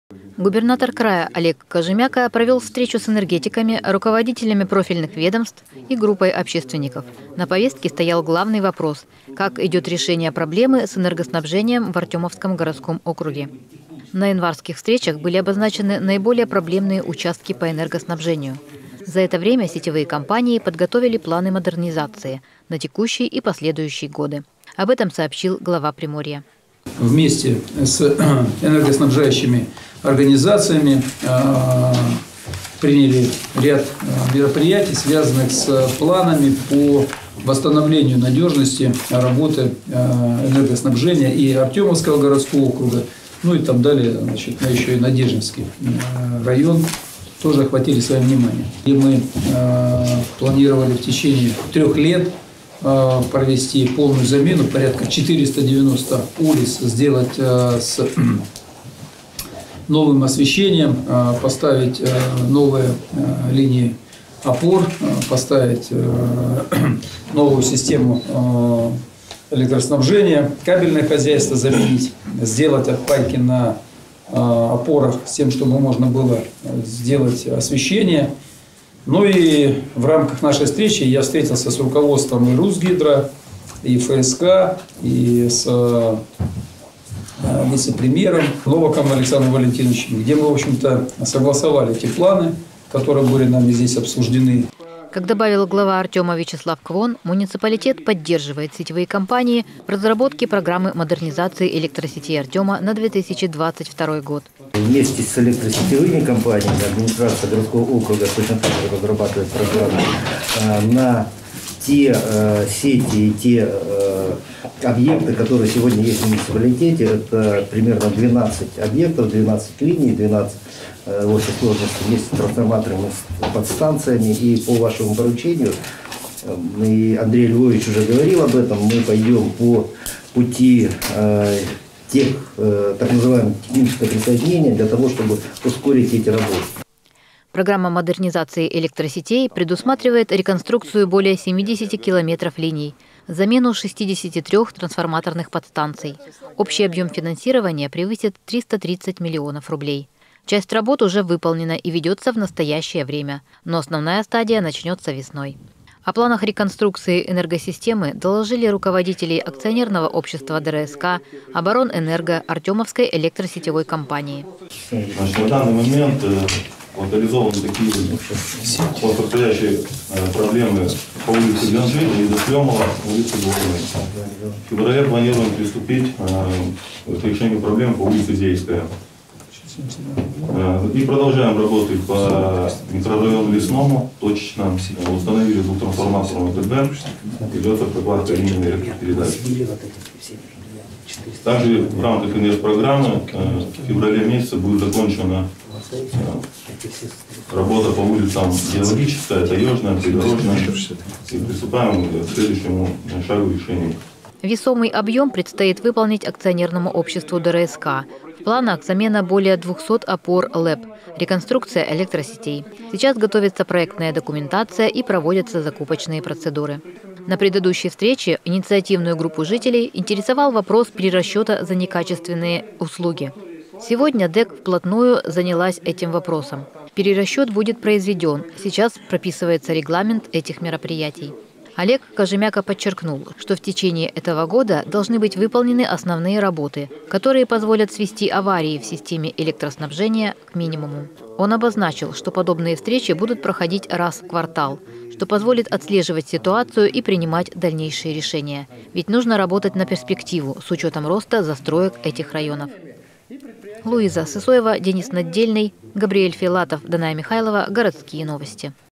Модернизация сетей электроснабжения Артёма идет полным ходом
Мы в Одноклассниках - https:/ok.ru/lentatv